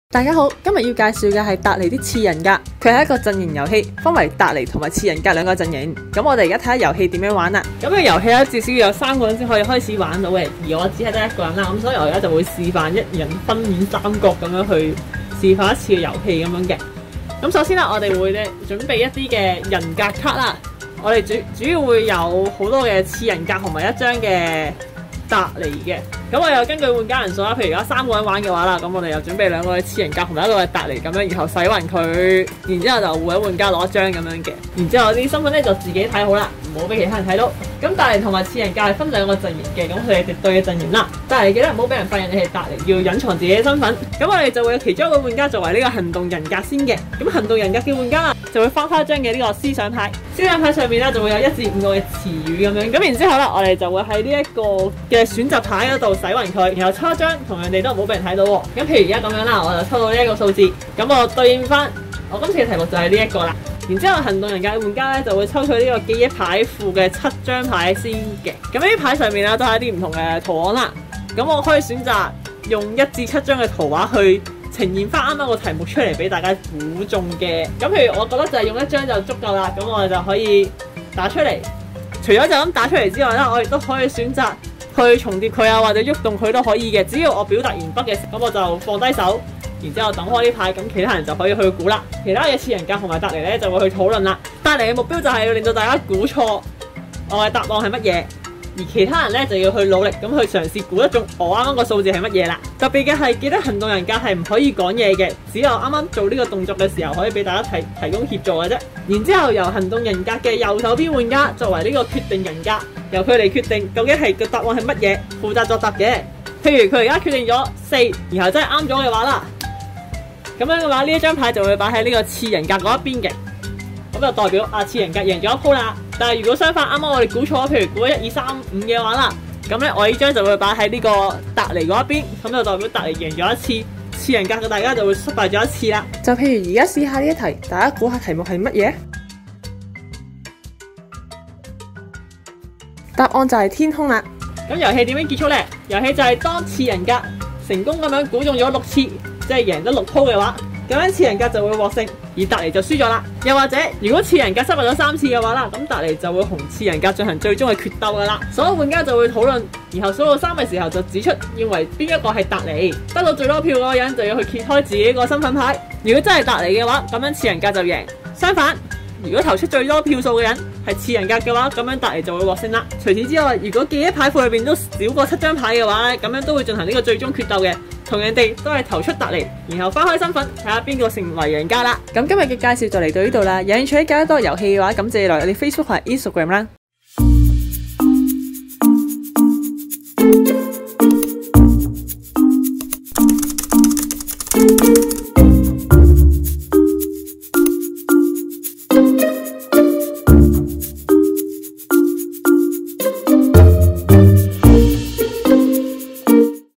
【玩完包你懷疑人生】潮流遊戲試玩 - 達尼的次人格
詳情:
玩家扮演達尼嘅其中一個人格👥👥
次人格為咗繼續存在必須通力合作,防止主人格搶回控制權
遊戲每回合由其中一個人格以圖卡勾勒題目
其餘人格溝通討論得出答案🗣
而主人格必須令次人格無法妥善交流,得出錯誤答案👅
但小心不要被發現.